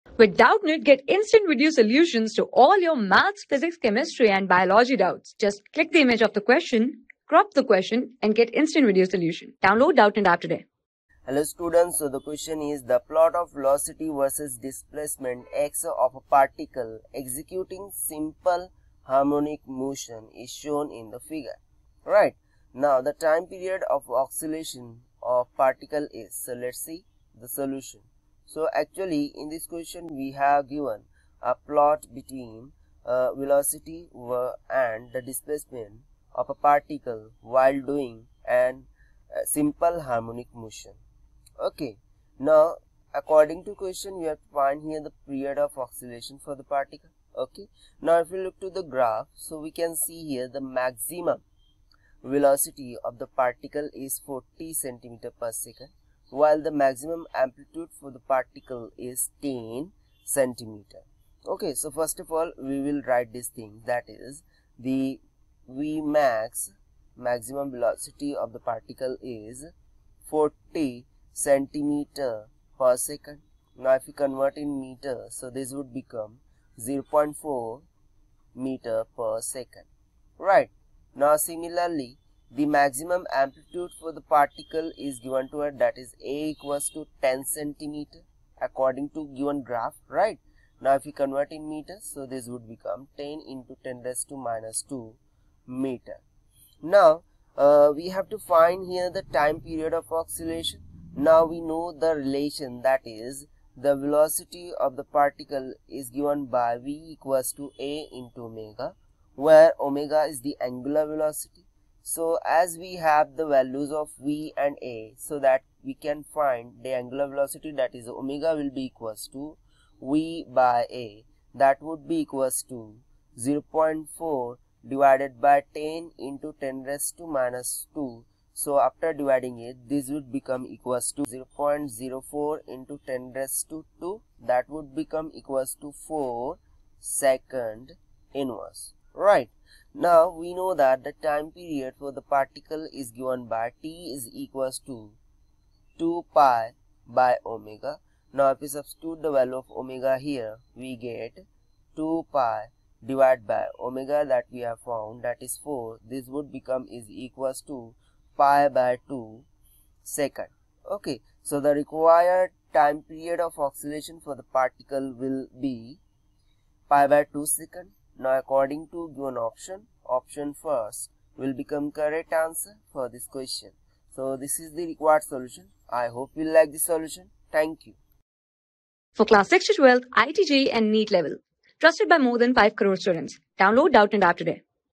The plot of velocity (v) versus displacement (x) of a particle executing simple harmonic motion ...
The plot of velocity (v) versus displacement (x) of a particle executing simple harmonic motion is shown in figure. The time period of oscillation of particle is :- \n
Class: 12
Subject: PHYSICS
Chapter: OSCILLATIONS
Board:IIT JEE

👉 Have Any Query? Ask Us.